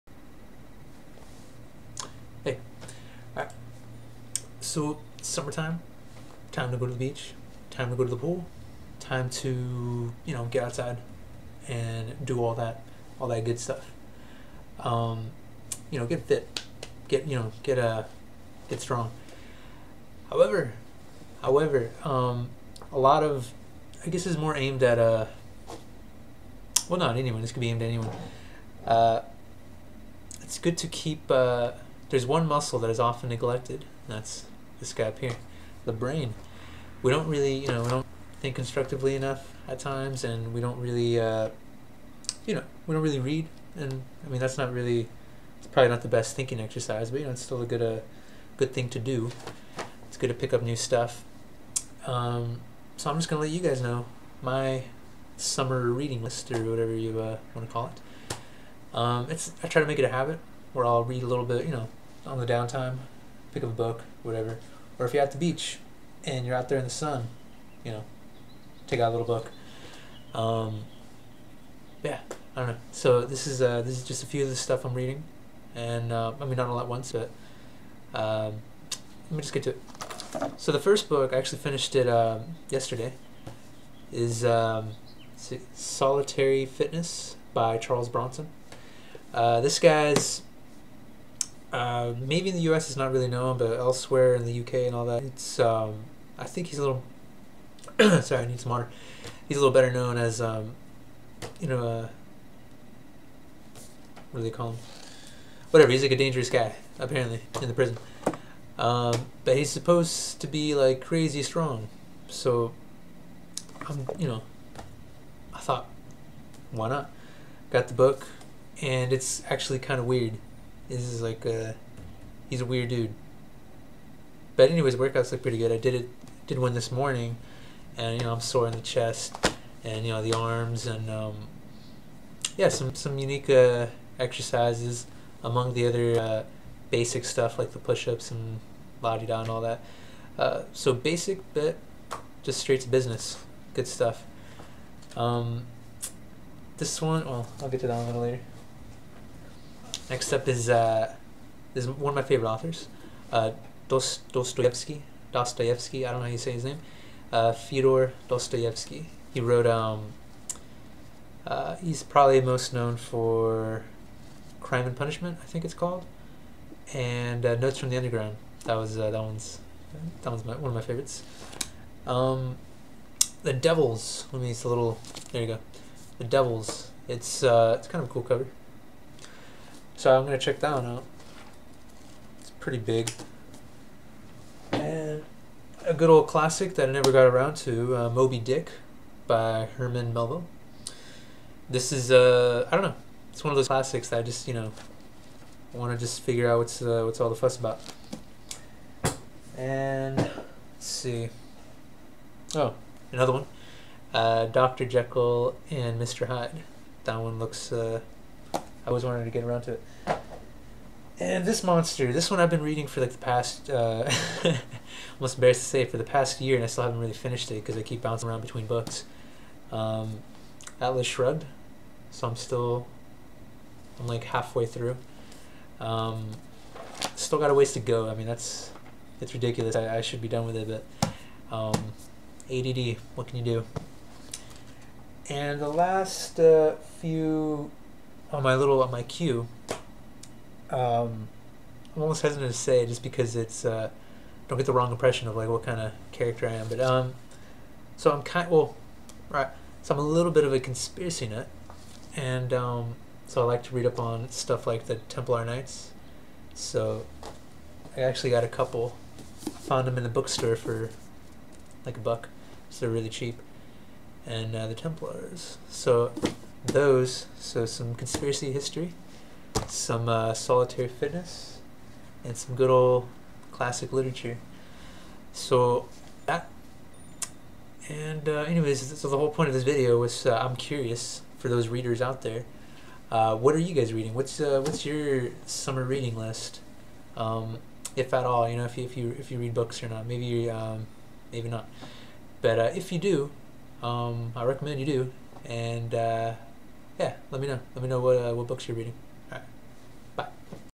Summer Reading List
Some books that I'm going to read during the summer.
Solitary Fitness- Charles Bronson
The Devils- Dostoyesky
Moby Dick- Herman Melville
Dr. Jeckyll and Mr. Hyde- Robert Louis Stevenson
The Templars: The History & Mytih- Michael Hagg
An illustrated history of the Knights Templar- James Wasserman